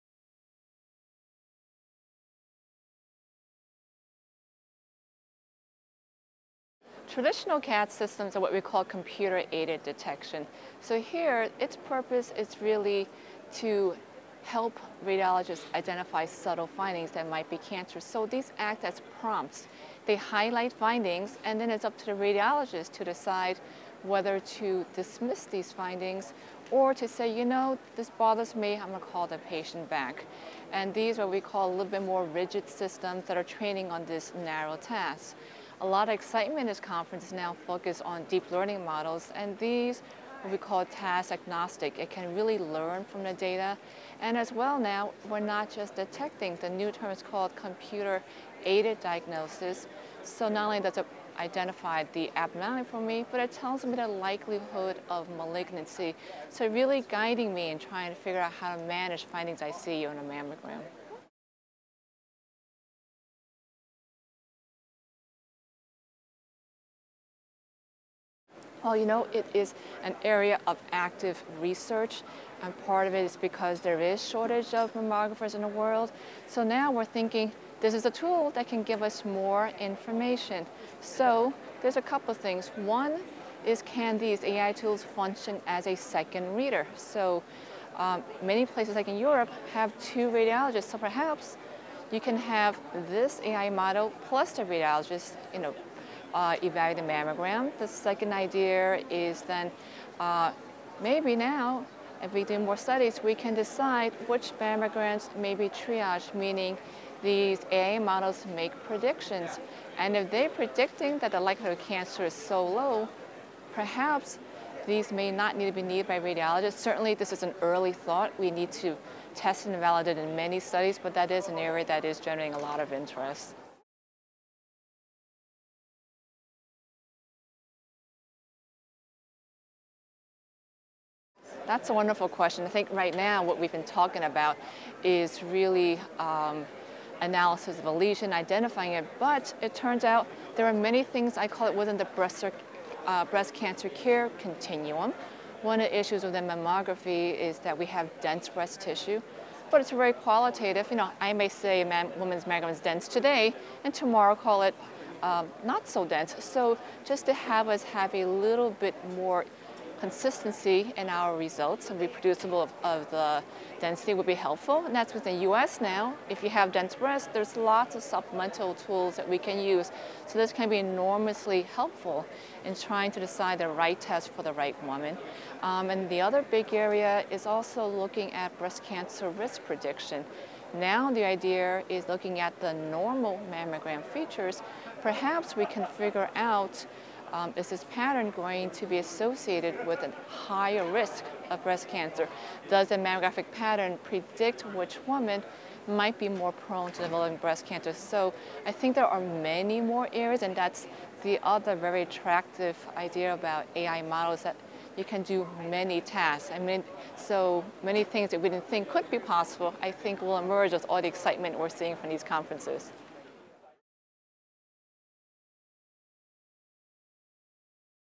Lessons Learned from CAD and AI
Linda Moy, MD discusses Lessons Learned from CAD and AI.